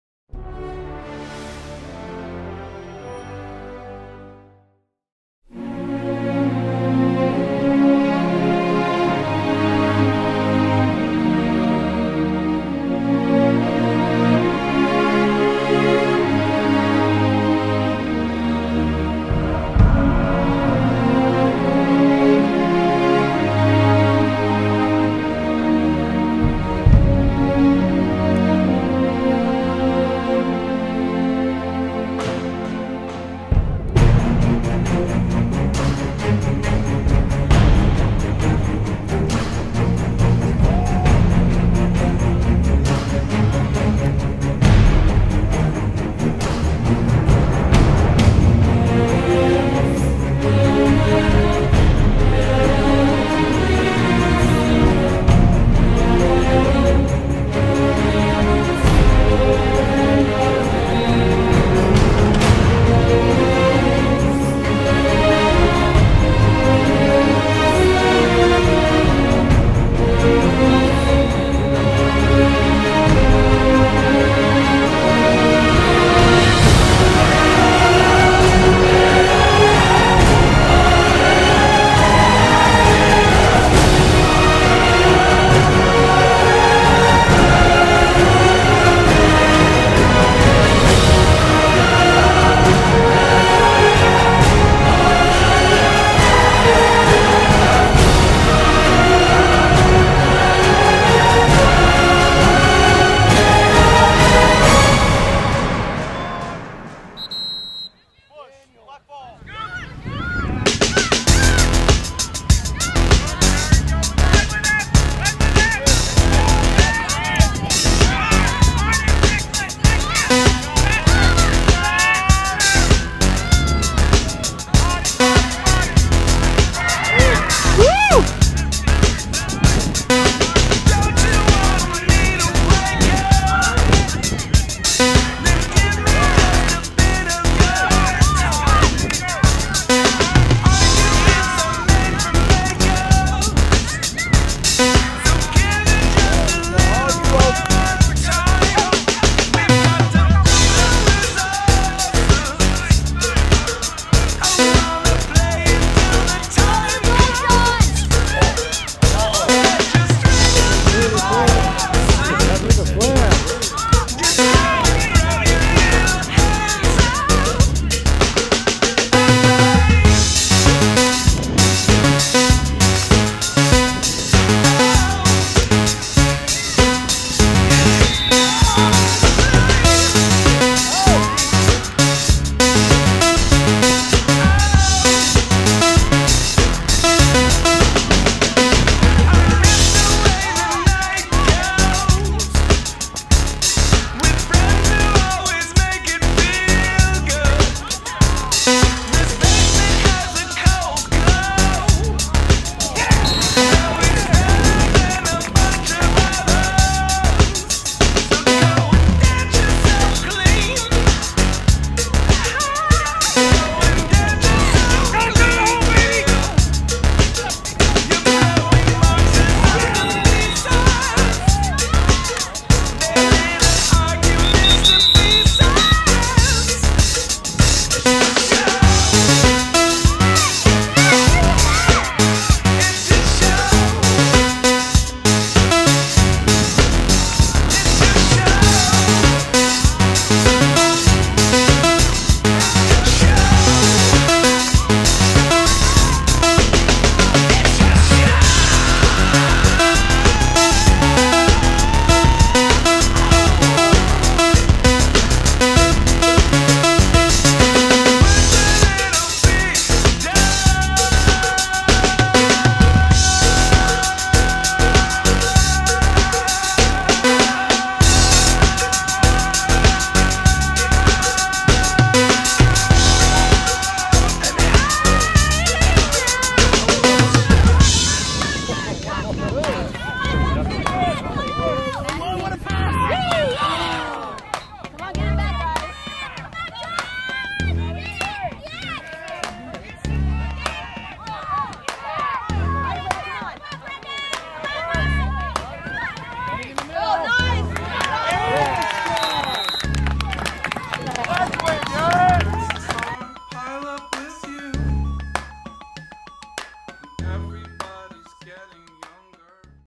John Morley Drury Oak Park Lacrosse U13 2013 Highlights
John Morley Drury #41 Game Highlights 2013 First year player.